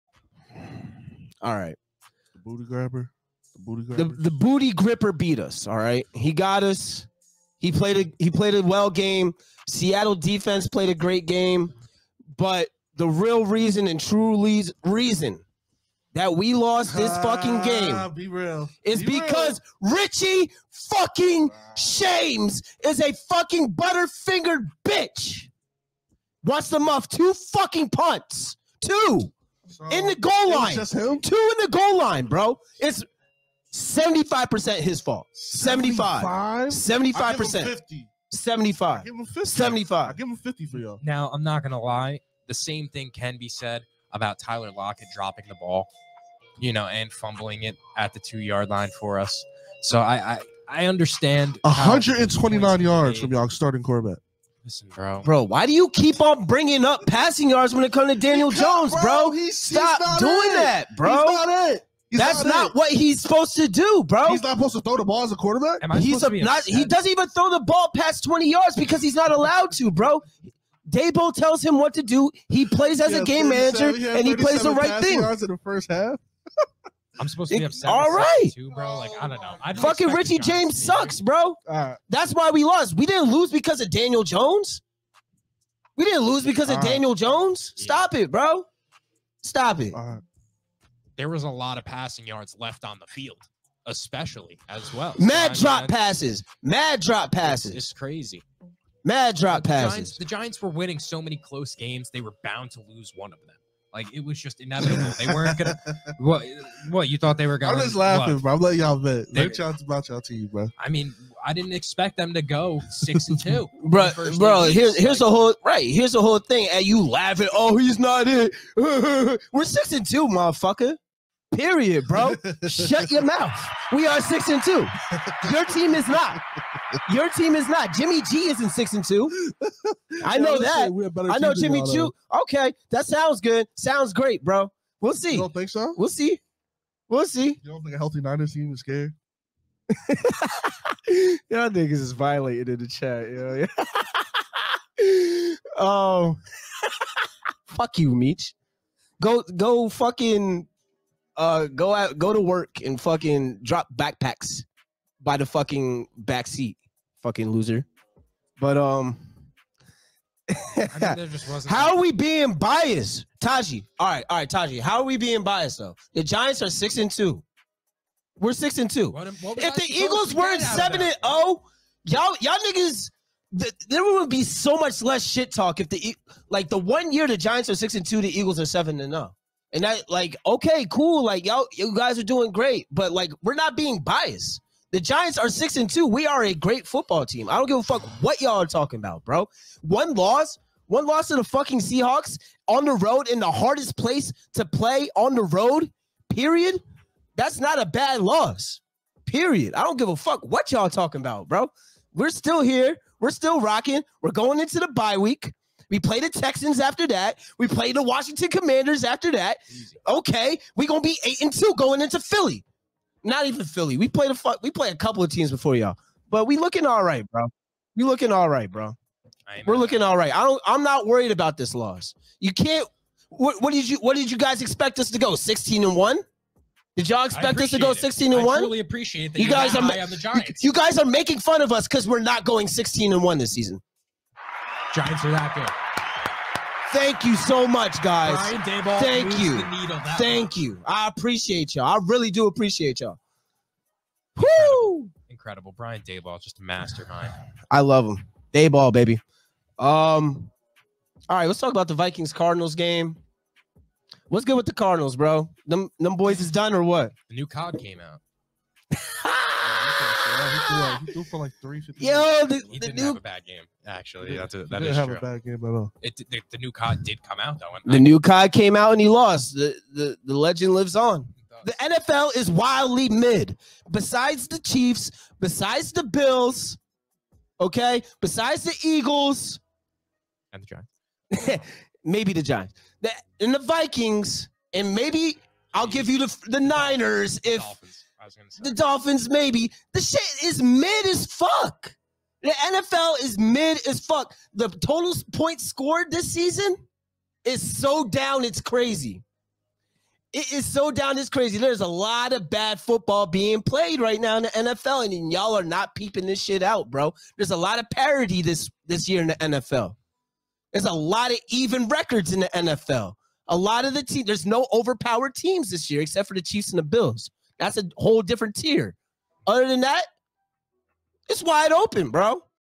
seahawks beat the giants
the Giants fell to the Seahawks in Seattle on Sunday. What went wrong for New York? Mo Sports TV Crew CMO,  Matt Chi, and Ed discuss.